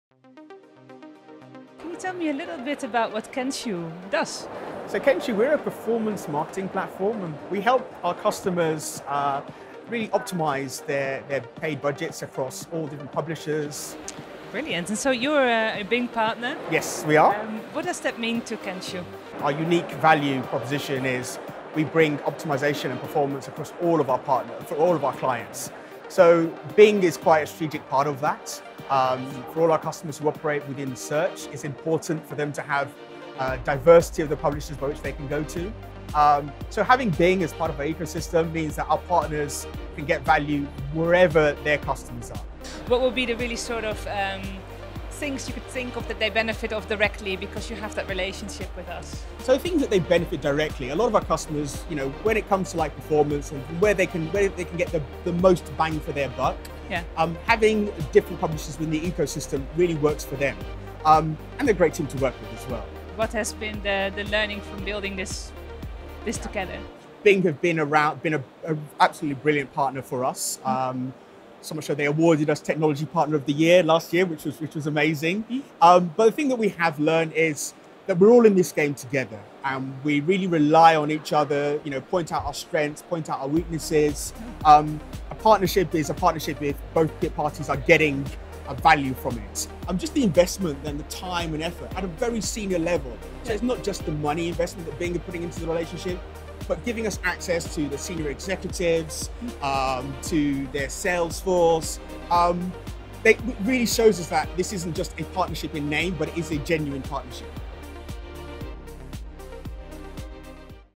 Interview with Kenshoo on the value of being a Bing Partner
We had the chance to interview Eb Adeyeri from Kenshoo on the value of being a Bing Partner.

Want to learn more about the Bing Partner program?